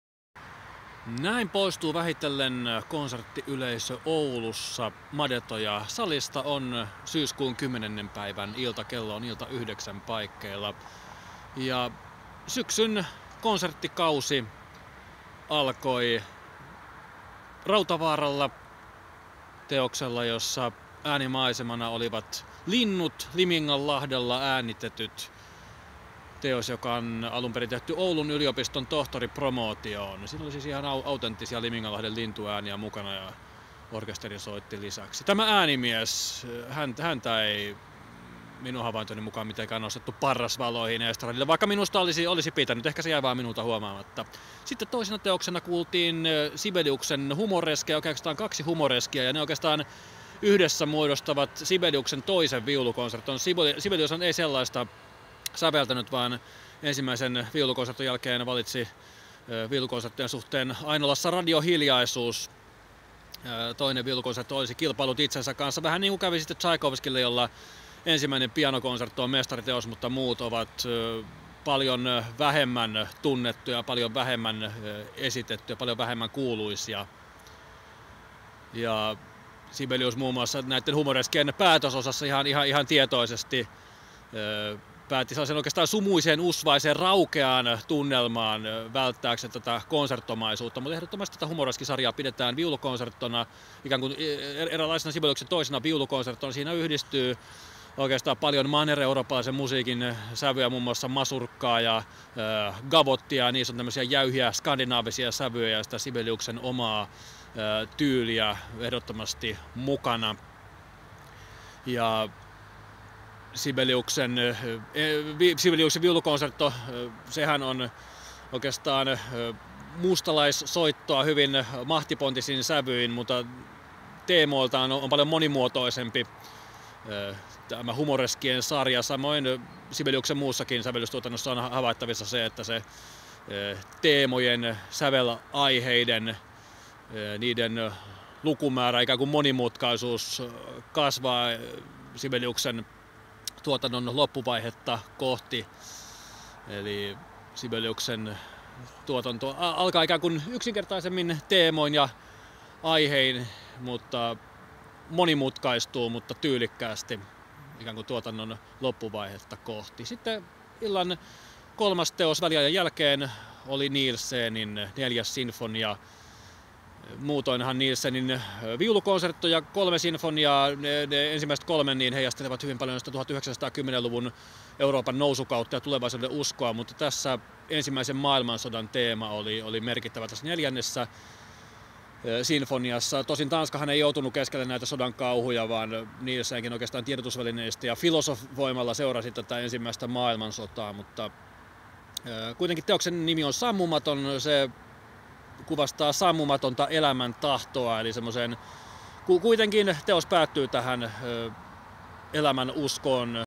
Oulu Sinfonia - konserttikauden avaus
Madetoja sali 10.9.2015 Rautavaara Nielsen Sibelius klassinen musiikki